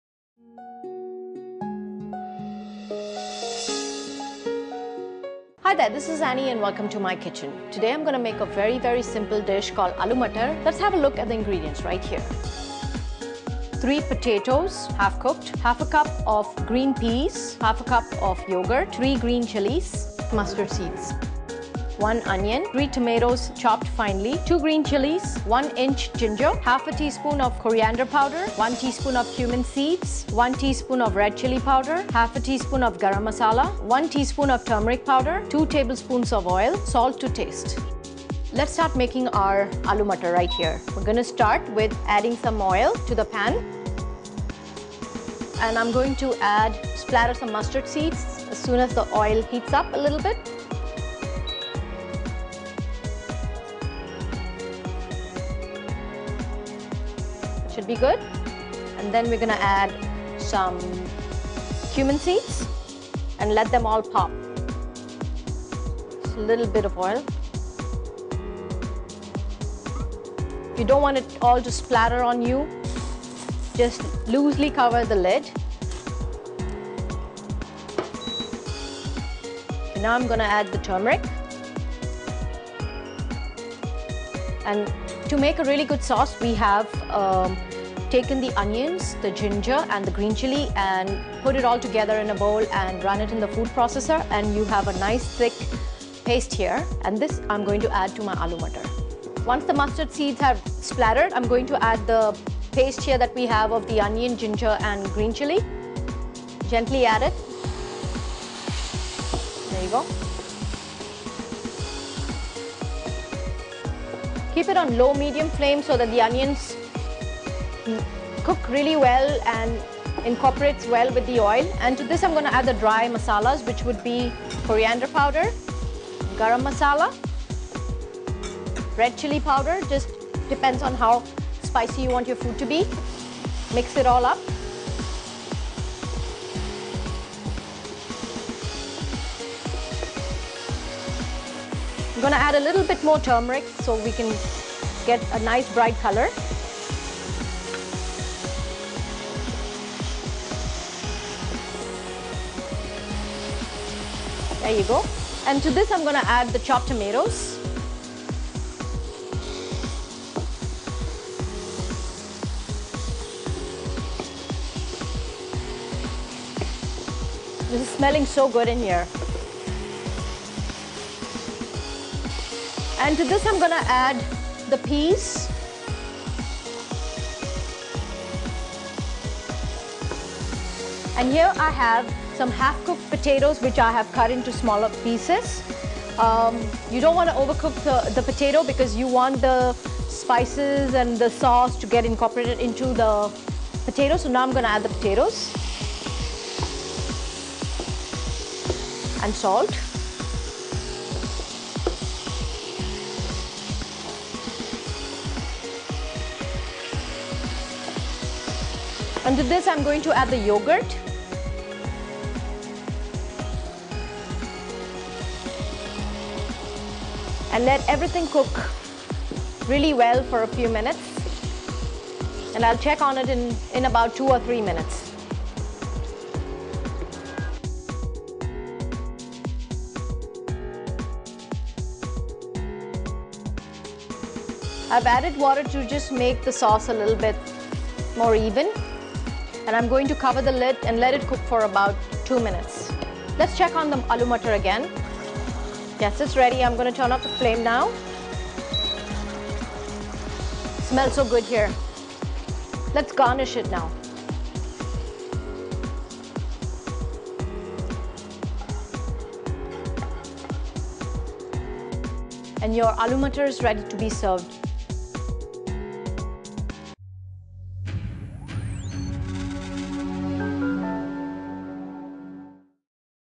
Aloo Mutter
Ingredients -
3 potatoes half cooked
1/2 cup green peas
1/2 cup yoghurt
1 tsp mustard seeds
1 onion
3 tomatoes
2 green chillies
1 inch ginger
1/2 tsp coriander powder
1 tsp cumin seeds
1 tsp red chilli powder
1/2 tsp garam masala
1 tsp turmeric powder
2 tbsp oil

Preparation -
Heat the oil.
Add the mustard seeds, cumin seeds, turmeric powder and mix well.
To make a paste - onion, ginger, green chilli and grind it.
Add the paste to the pan.
Add the dry spices to the pan - coriander powder, garam masala, red chilli powder and mix well.
Add the tomatoes, peas, potatoes, salt, yoghurt and let it cook.
Add water.
Serve hot.
Garnish with carrot sticks.